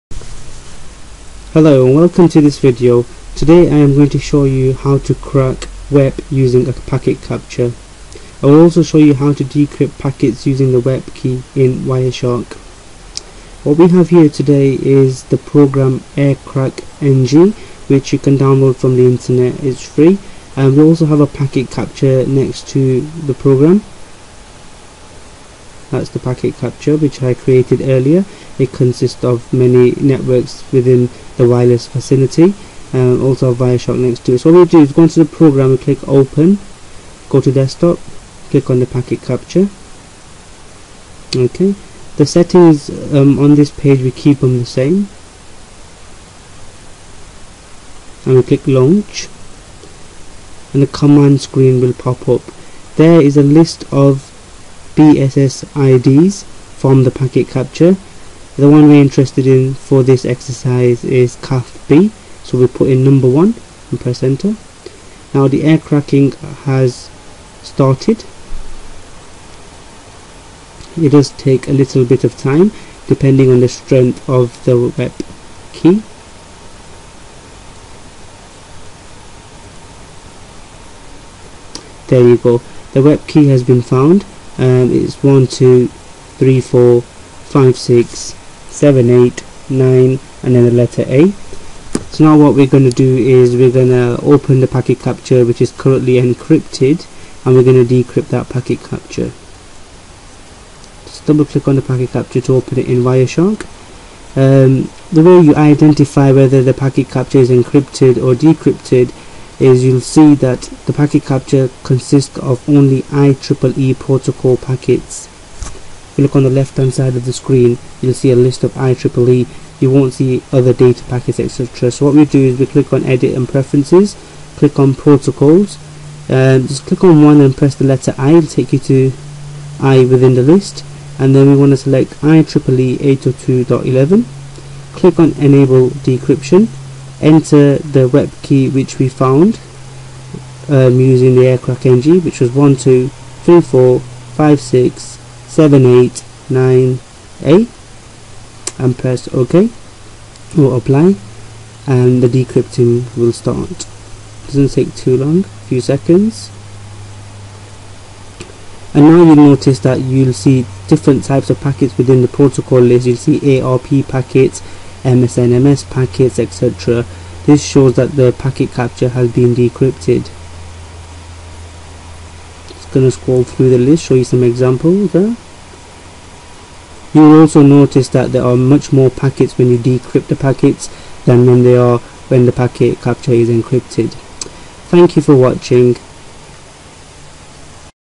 Wireless Forensics 1
This video shows you how to crack WEP using a packet capture and the tool Aircrack-NG, the video also shows you how to then decrypt the content of the packet capture within Wireshark.